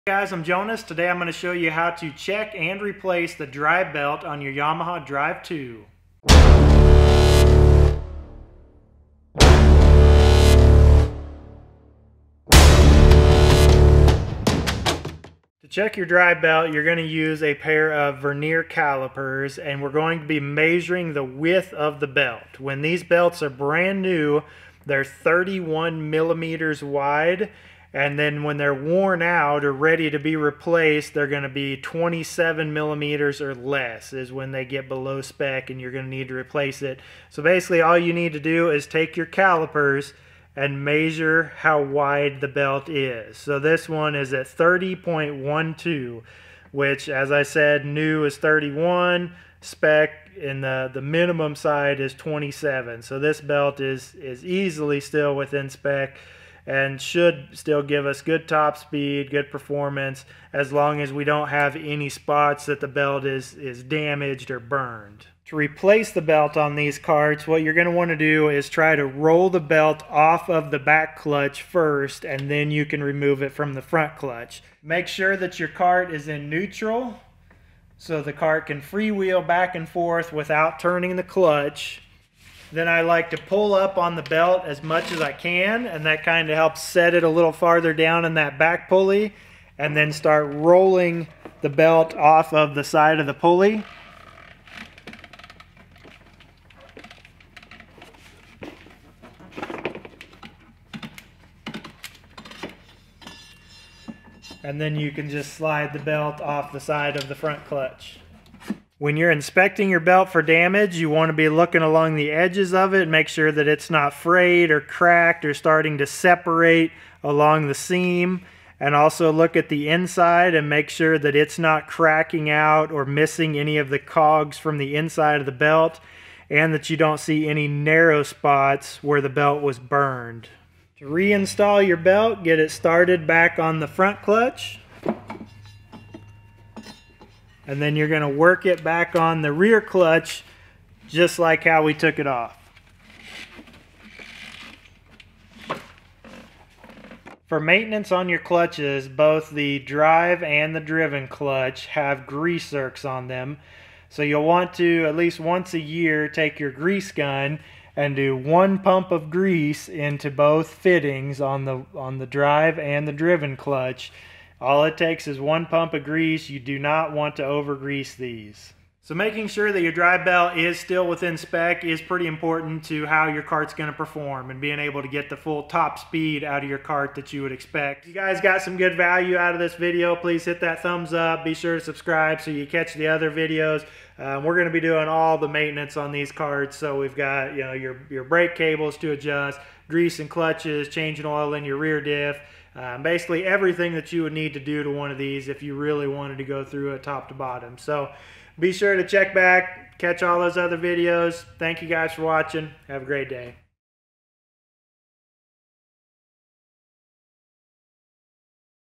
Yamaha Drive 2 Quietech G29 Drive Belt Replacement
This video covers how to check and replace the drive belt on your Yamaha Drive 2 G29 golf cart. The spec for this belt is 31mm new and if it is below 27mm it needs to be replaced.

Mini grease gun for clutches